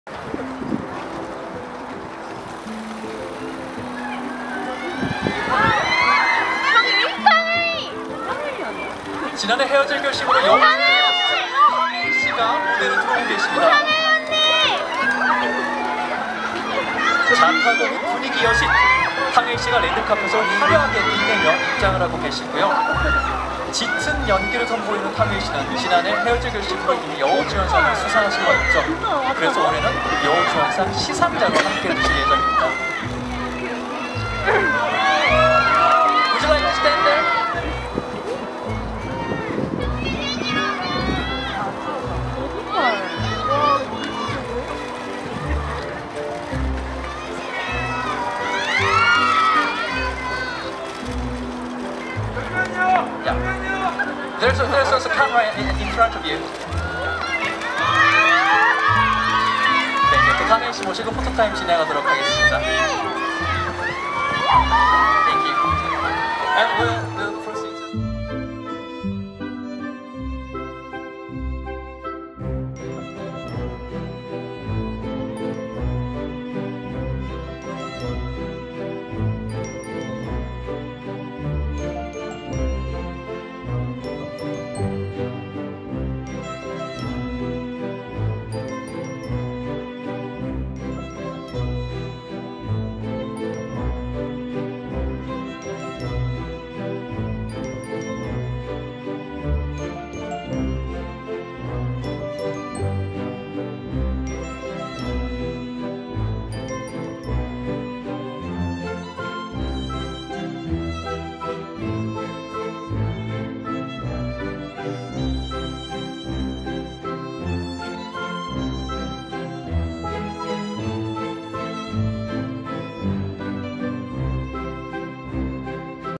탕웨이(汤唯 | Tang Wei) 시상자로 깜짝 등장 (청룡영화상 레드카펫) | BLUE DRAGON AWARDS RED CARPET • 231124
▶︎ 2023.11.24 Blue Dragon Awards Red Carpet @여의도 KBS
▶︎ 탕웨이(汤唯 | Tang Wei) 시상자로 깜짝 등장 (청룡영화상 레드카펫)